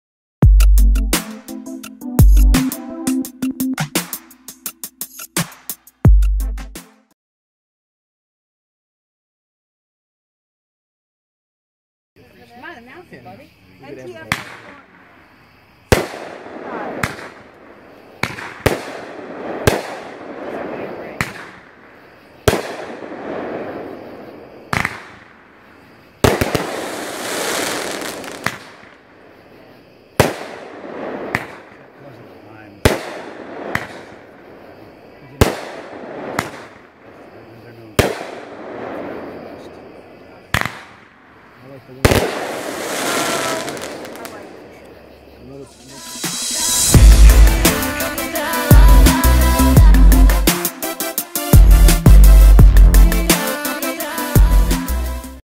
Fireworks Demo (500 Gram Cake) - NTF-1 (Black Cat)
Unfortunately, I could not find a picture of this item, so I had to go with a video thumbnail.  This cake has some nice breaks and it uses the yellow/lemon color.